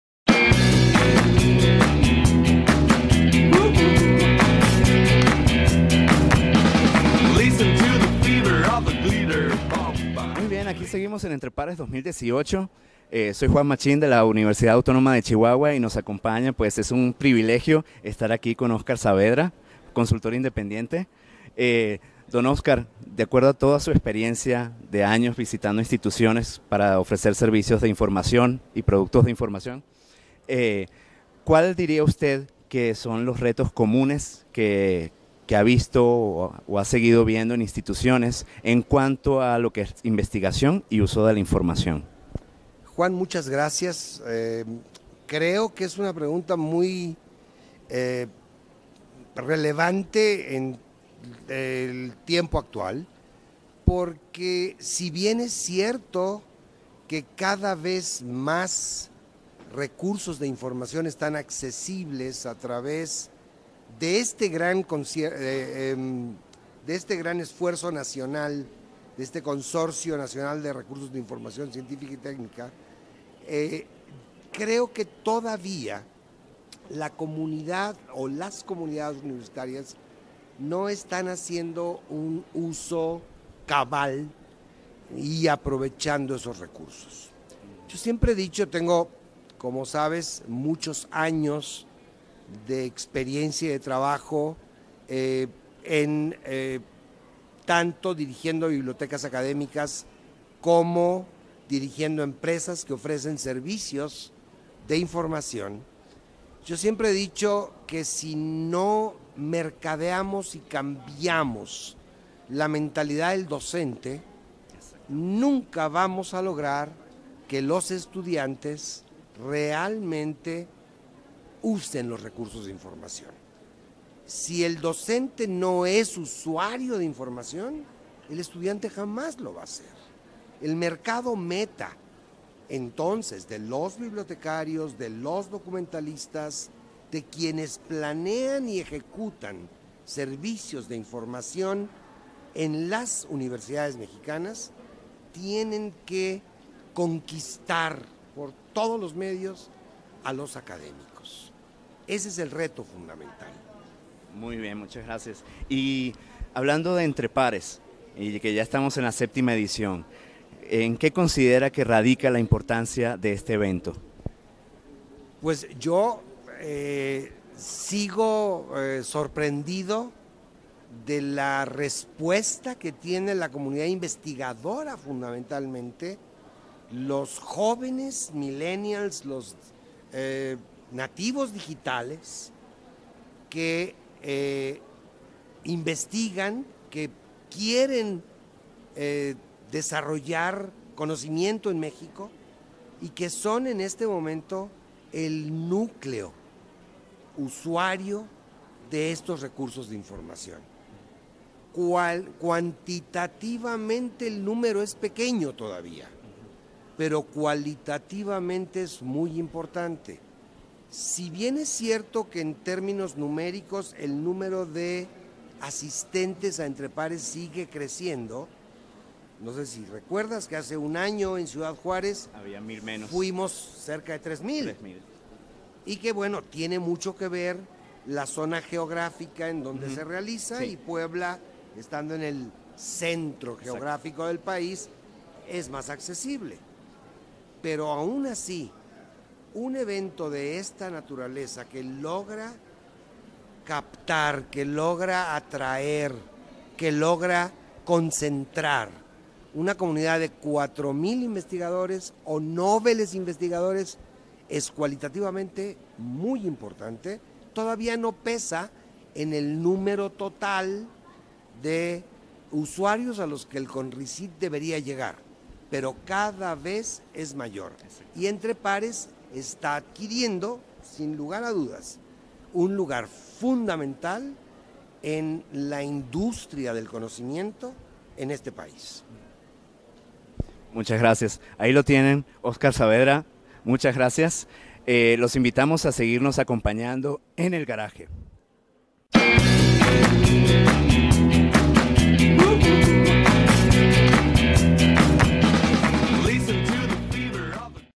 Investigación e información - Entrevista con Oscar Saavedra
Hablamos con el gran Oscar Saavedra, consultor independiente, sobre los retos en cuanto a investigación y uso de la información en las instituciones educativas y de investigación.